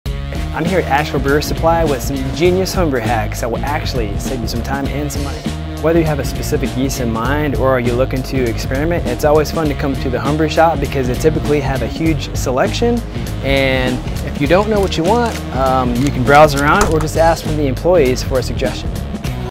Shopping Hacks For Your Local Brewery Supply Store.
Looking for your next yeast strain? Try heading down to your local brewery supply store.

For us at Clawhammersupply most brew days start with a trip to @asheville.brewers.supply for ingredients & updates on the brewing industry.